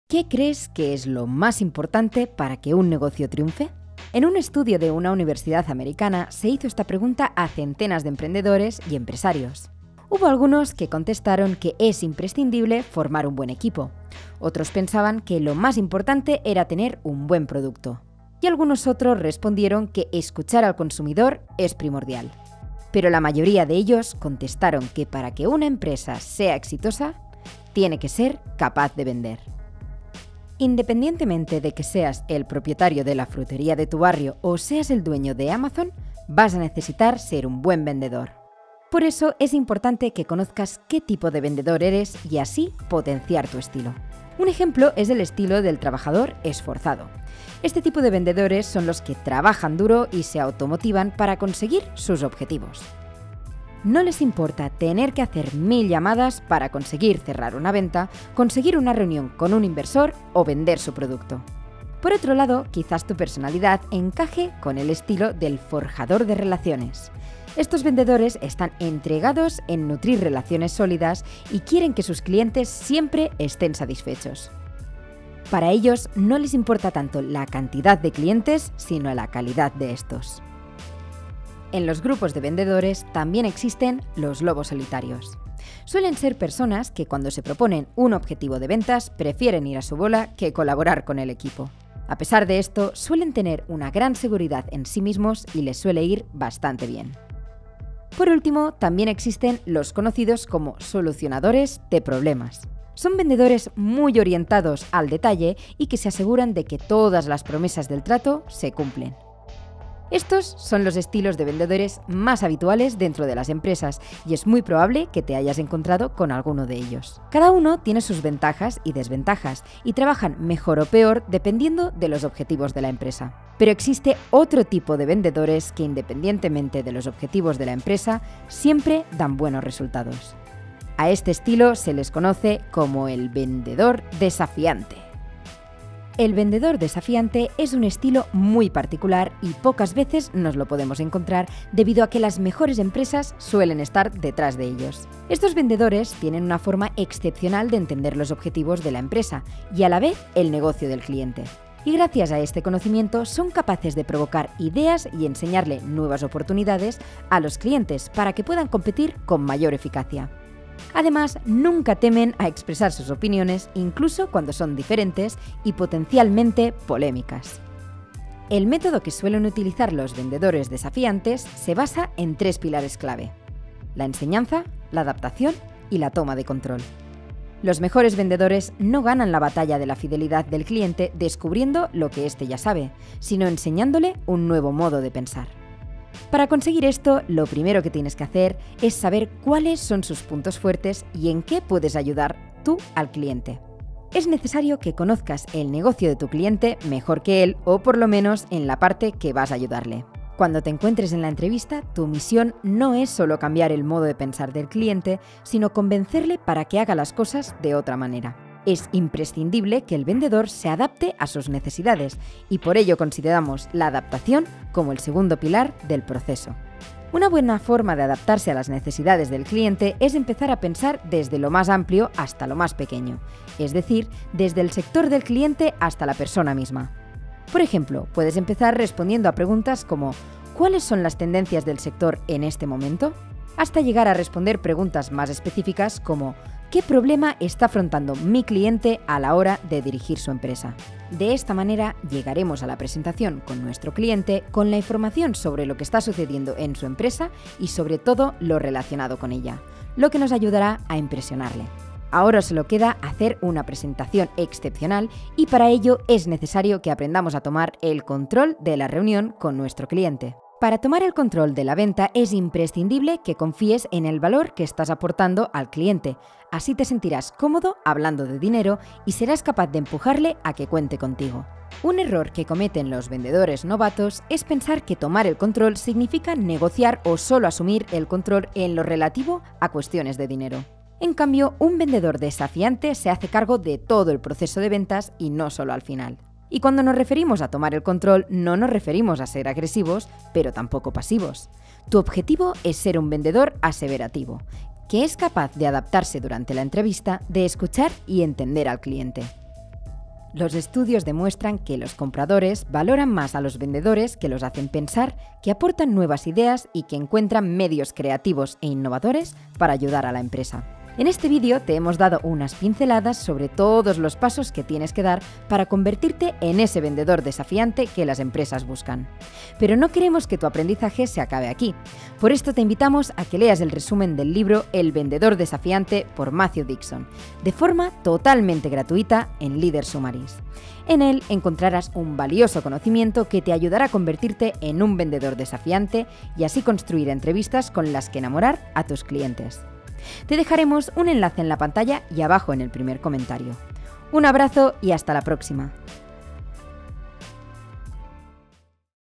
💰¿Qué tipos de vendedor existen y consiguen más clientes? | El Vendedor Desafiante Por Matthew Dixon
¿Quieres saber los tipos de vendedores que existen y cómo puedes vender más? En este vídeo hablamos sobre el libro: El Vendedor Desafiante, por Matthew Dixon, y de los métodos utilizados por los mejores vendedores para conseguir clientes.